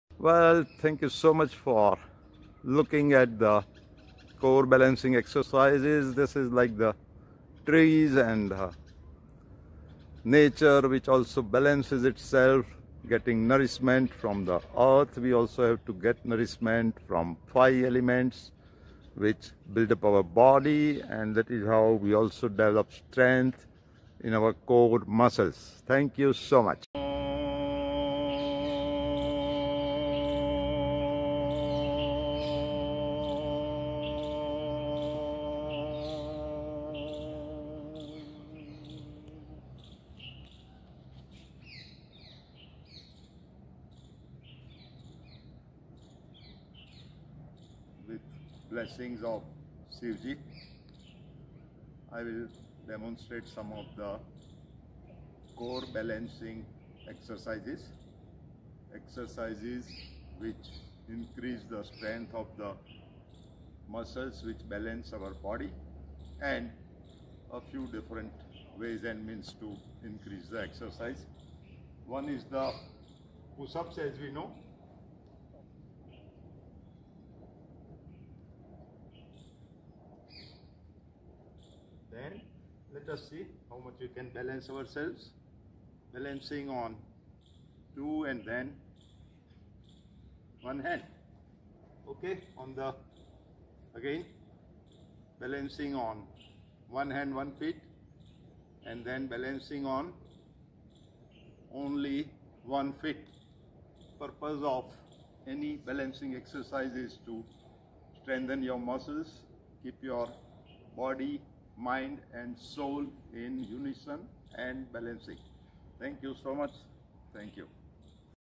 Core strengthening, Core balancing, Having Joyous life during Corona Times.
Dr Hemant Chimanlal Parikh, ( , , ), MD DGO LlB Yog Teacher, Bliss Coach; from Gujarat, India, shares moments of core balancing and core strengthening. In corona times, it is important to strengthen your chest and respiratiory muscles. By strengthening your core muscles, you become stronger and more immune. Remain careful, while performing alone! JOYOUS and Safe life!!